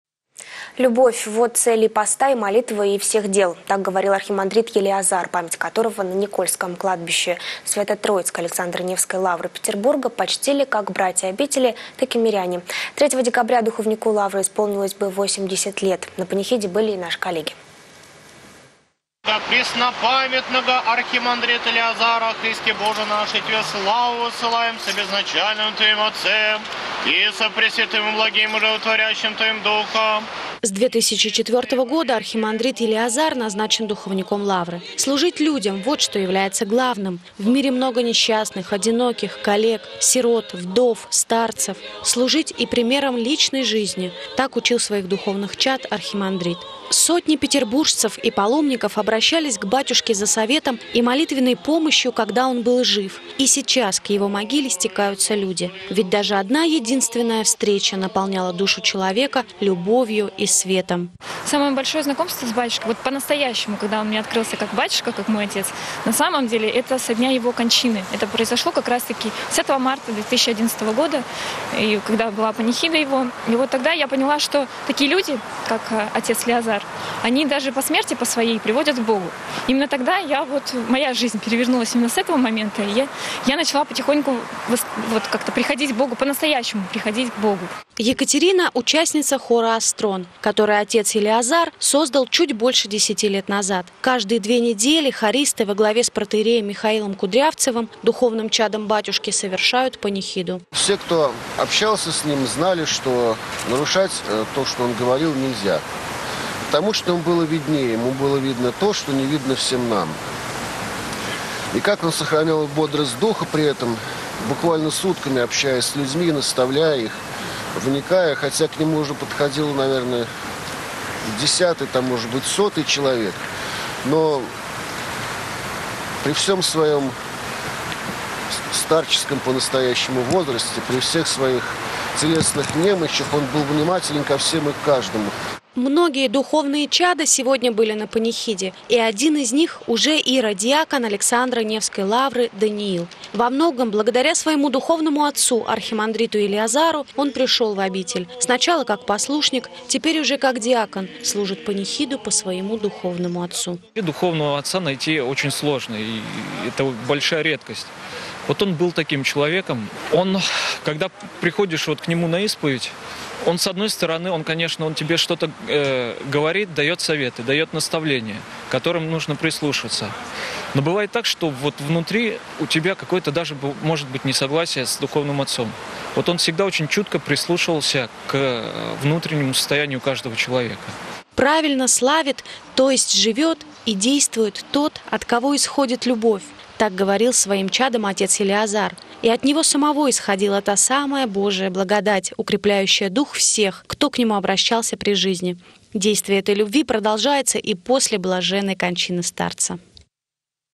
На Никольском кладбище Свято-Троицкой Александро-Невской Лавры почтили память архимандрита Елеазара
Любовь - вот цель и поста, и молитвы, и всех дел. так говорил Архимандрит Елеазар, память которого на Никольском кладбище Свято-Троицкой Александро-Невской Лавры Петербурга почтили как братия обители, так и миряне. 3 декабря духовнику Лавры исполнилось бы 80 лет. На панихиде были и наши коллеги.